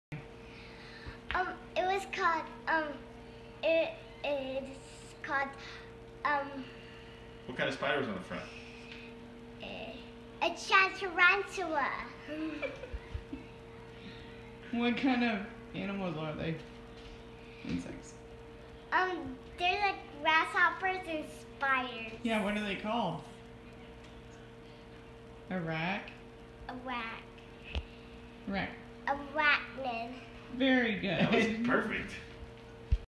Little Big Phenom says tarantula really cute
Loralei is learning about spiders in Kindergarten at school and she has a cute way of saying tarantula